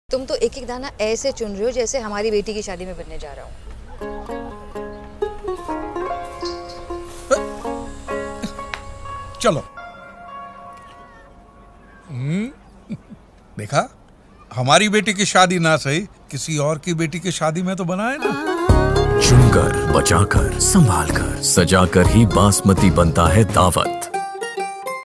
Daawat The Finest | Chunkar, Bachakar, Sambhalkar, Sajakar | Aise Banta Hai Daawat Sabse Khaas
Ab shubh avsar ko, aur bhi shubh banaayein Daawat Basmati ke saath!

Kyunki, Daawat Basmati ke lambe, khile-khile aur khushboodaar daane hum kheton se khaas chun kar laate hain, dhoop-barish se bachakar factory tak pahunchaate hain. Sambhal kar apni factory mein age karte hain aur phir aapko bade pyar se sajakar paroste hain. Tab jaake banta hai yeh India ka Finest Basmati Rice, the one that you deserve on your special day.